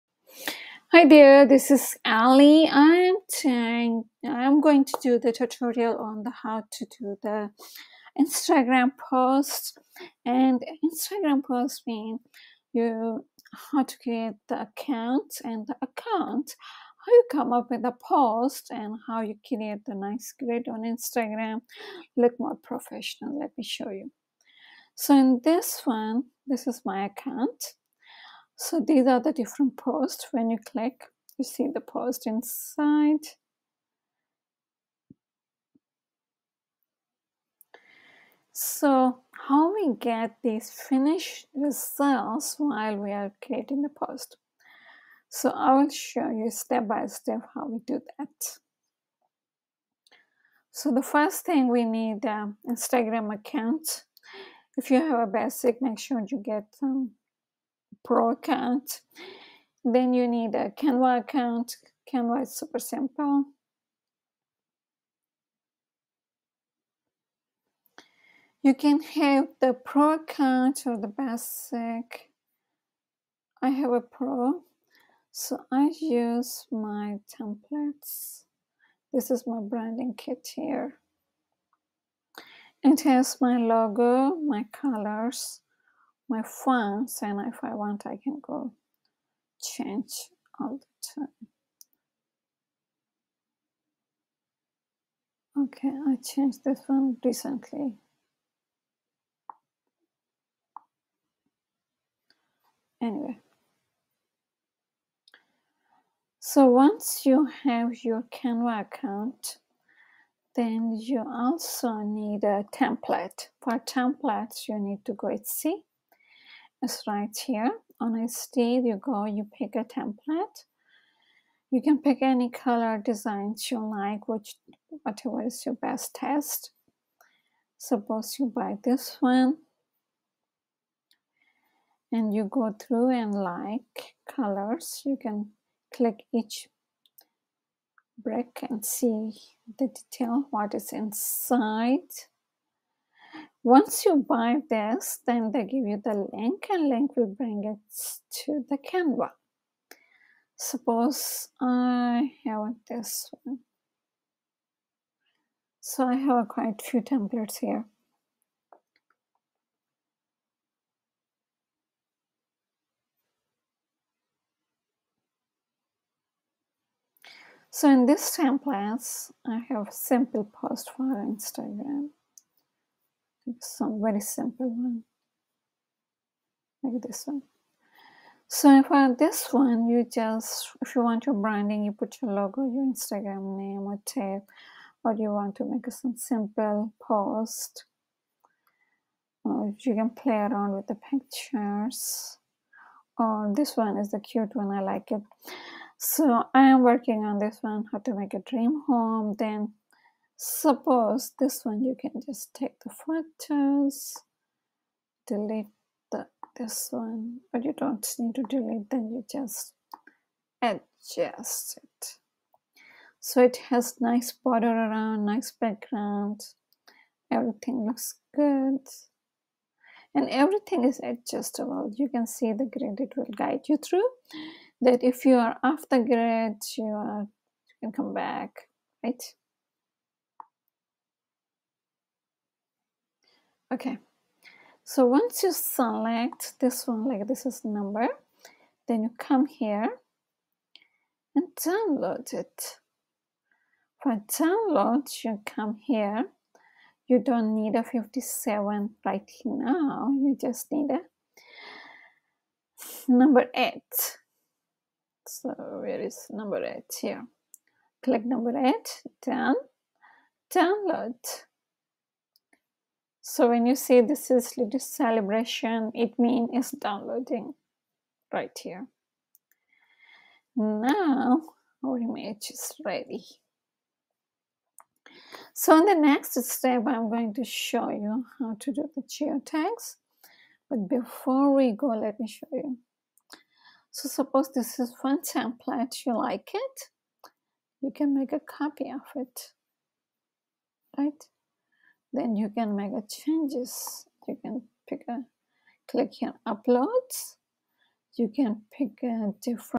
How to create an Instagram post in Canva
Tutorial on how to create an instagram post in Canva.

1 - How to create an Instagram post.
5 - Add keyword everywhere Extention in Chrome.